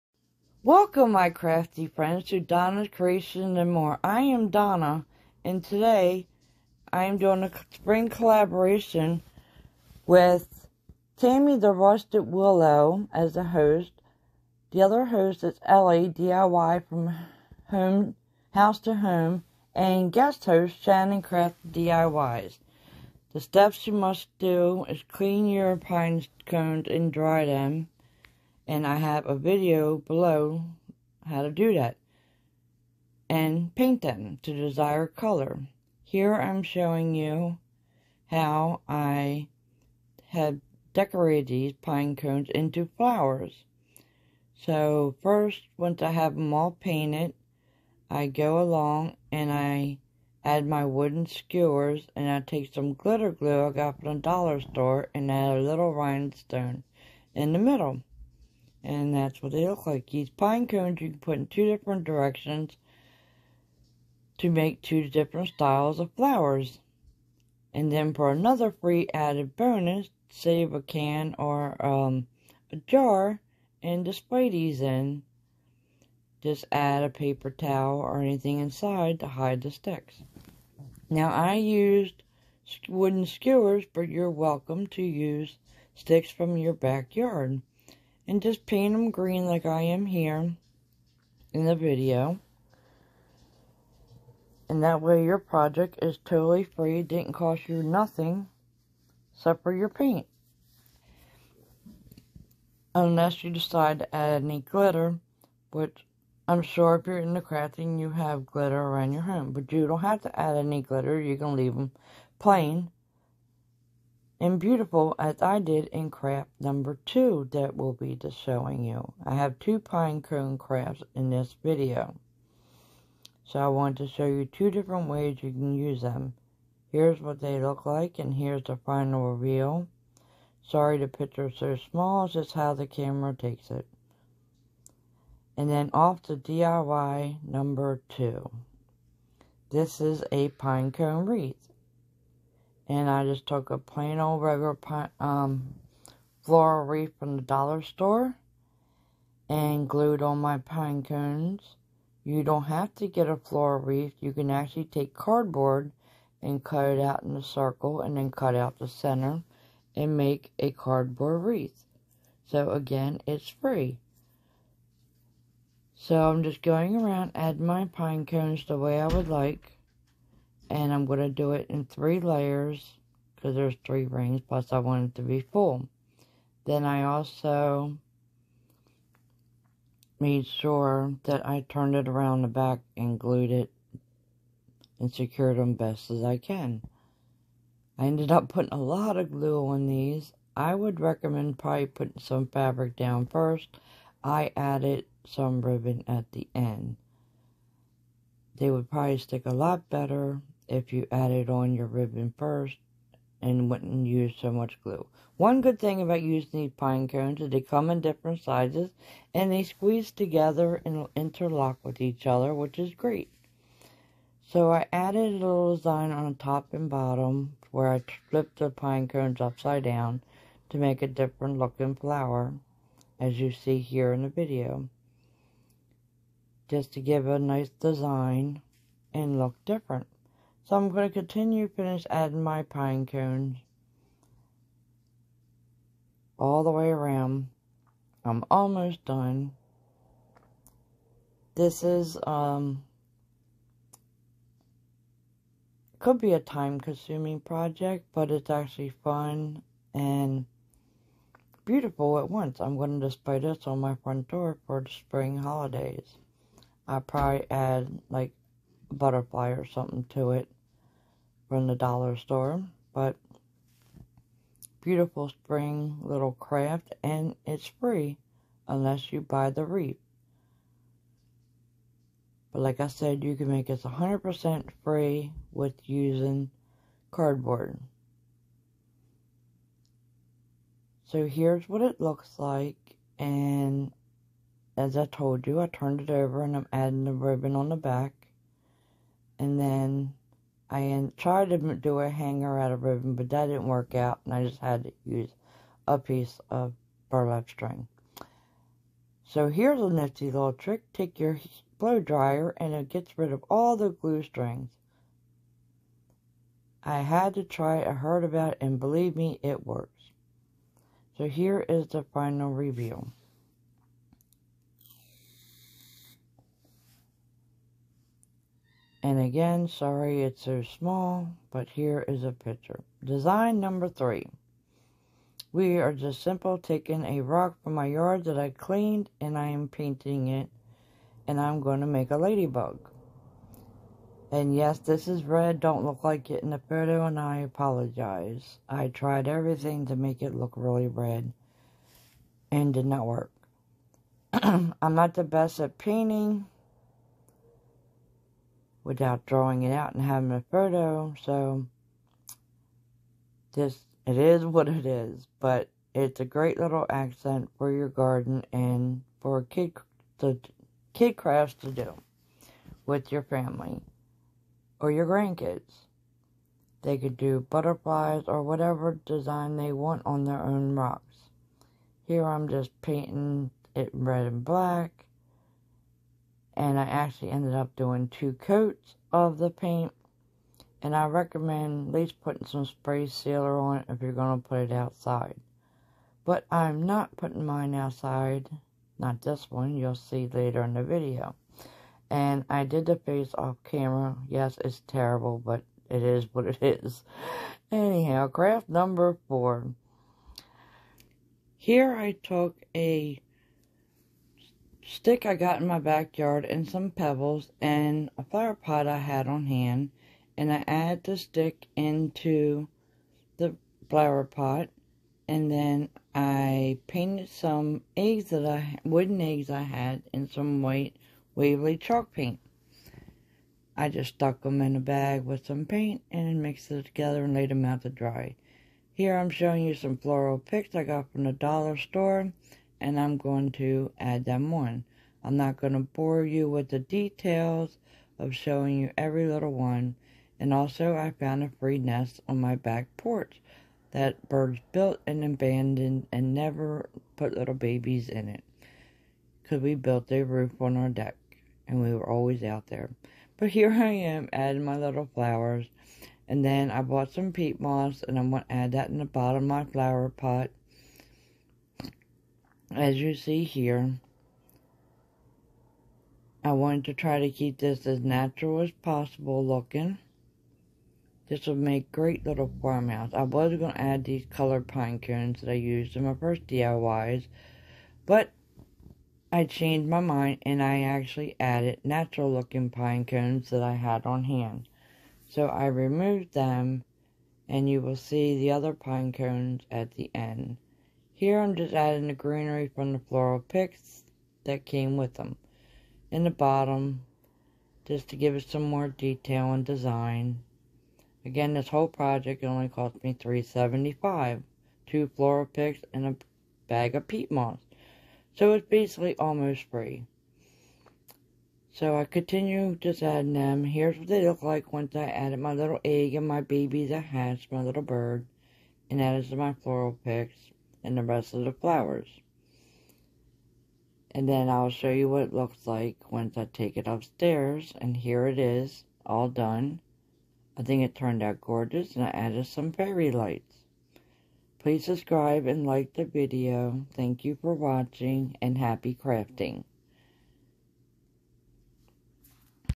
SPRING DIY CRAFTS 😃 FREE CRAFTS/ pinecone crafts
FREE SPRING CRAFTS FROM NATURE 5 MIN CRAFTS !!!
spring crafts kids can do too
PINE CONE CRAFTS, ROCK CRAFTS FOR YOUR GARDEN , AND YOUR VERY OWN LIGHTED FAKE TREE THIS BEAUTIFUL CHERRY TREE
yoduvh essentials
videos will be posted in slower motion. Sorry, I made this 1 speed up too fast
beautiful creations by Donna

@TheRustedwillow -Host Tammie
@shannoncraftydiys- Guest host
@Diyfromhousetohome -Host Ellie